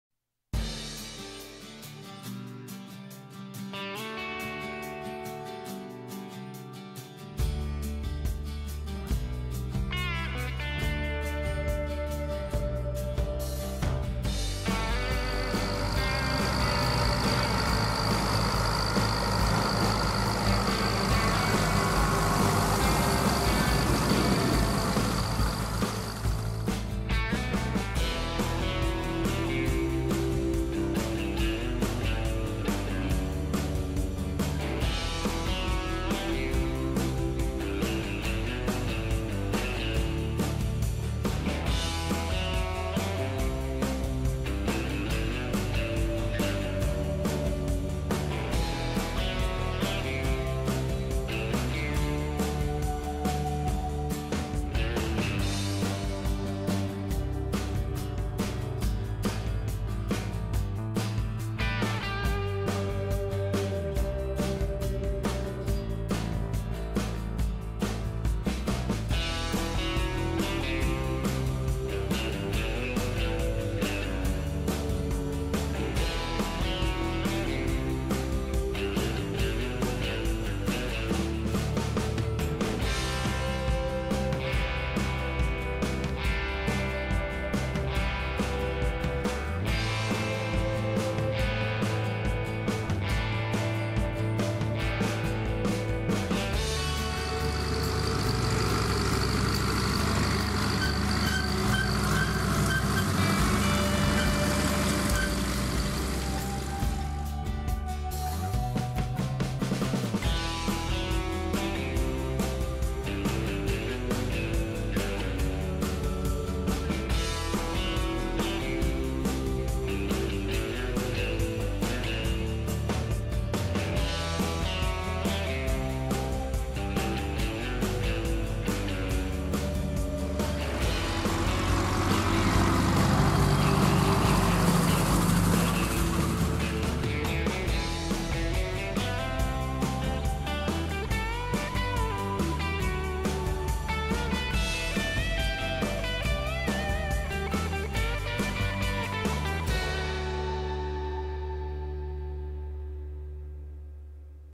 Raking grass | Fendt Farmer 306 Lsa | Vicon Andex 463 | Gras harken
-  Raking grass | Gras harken | Gras rechen
- Fendt Farmer 306 Lsa | Vicon Andex 463
- Bakel, Netherlands

Equipment
Panasonic HC-V180
DJI Phantom 3 SE
Action Cam